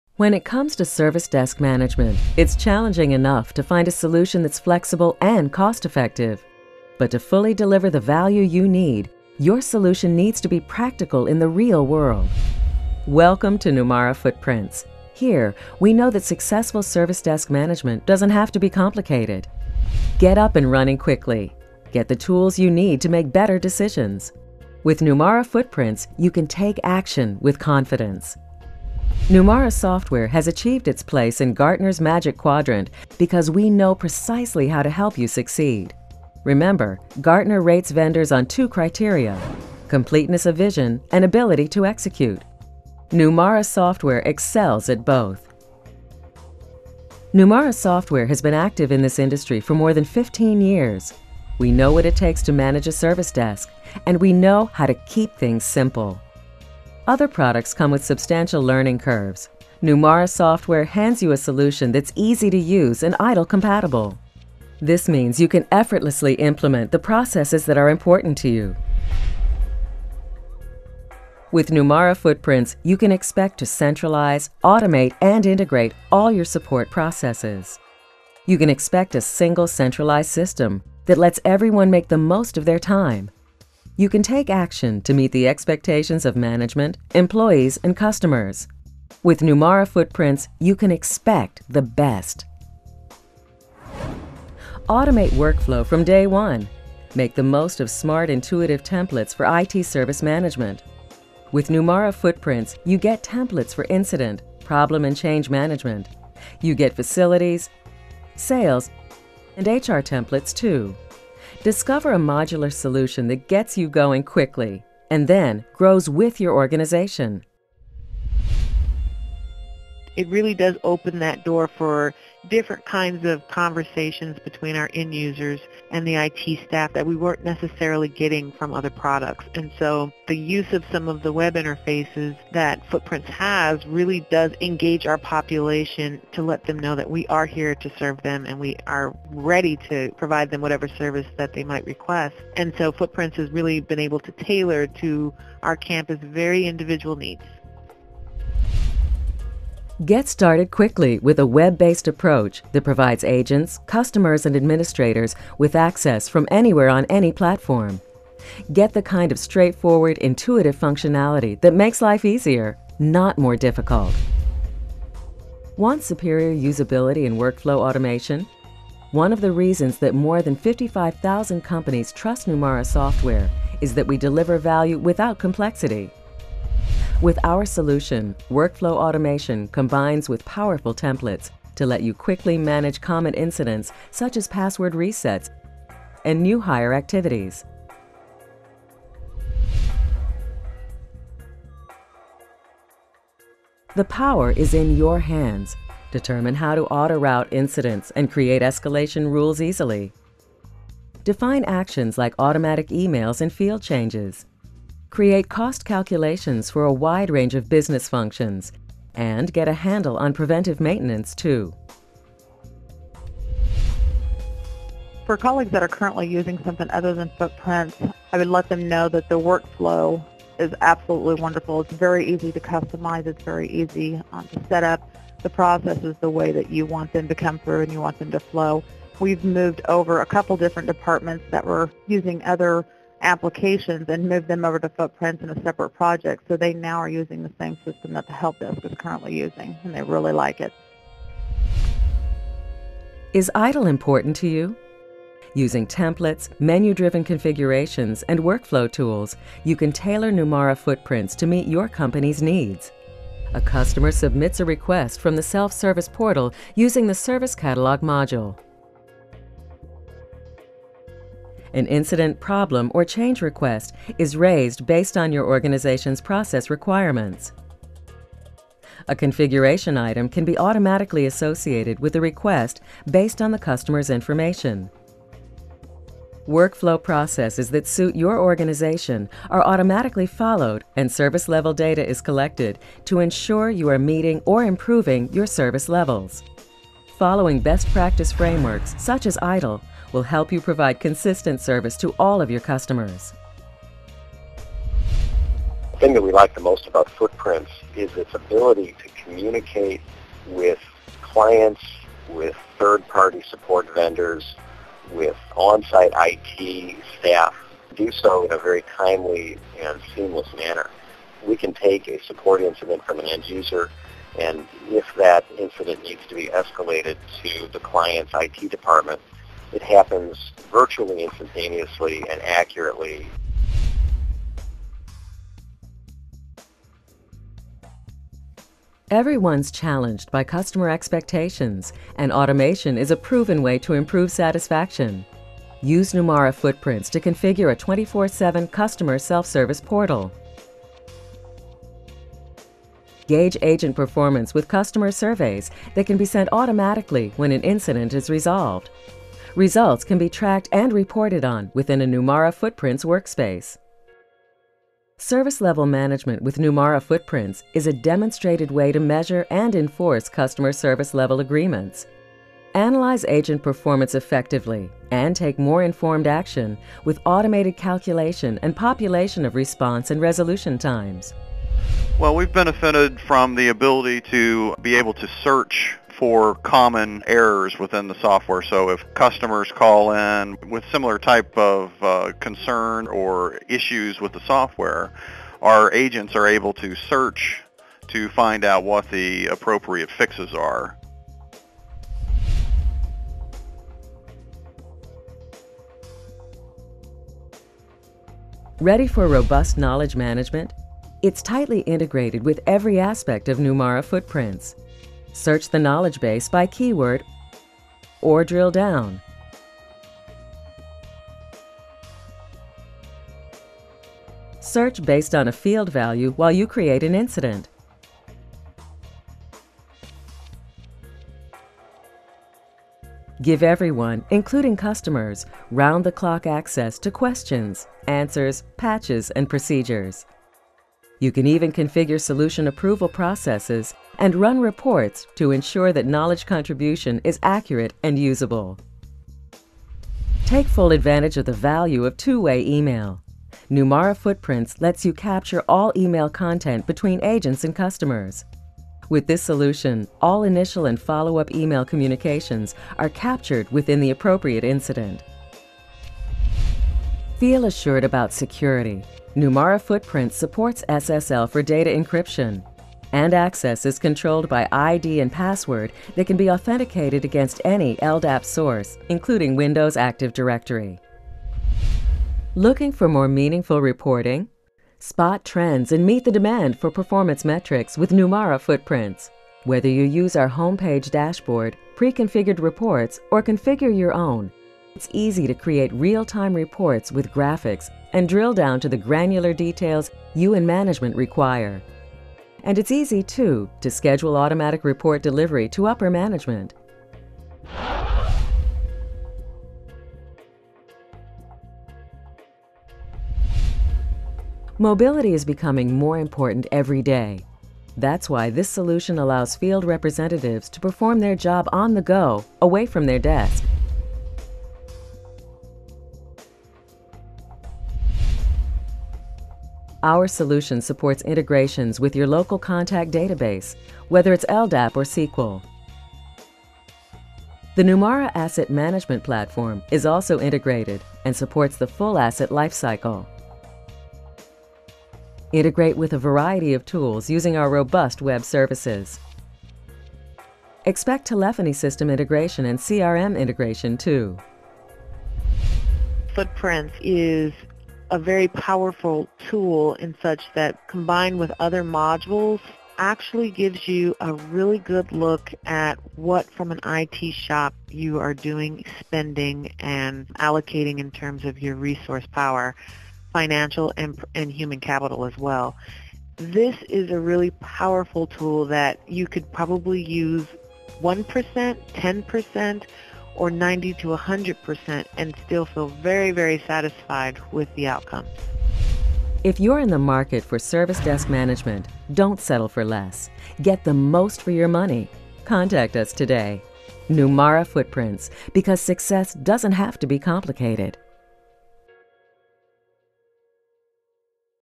Numara Software releases FootPrints 9 Service Desk
A quick overview of the service desk features of Numara FootPrints 9.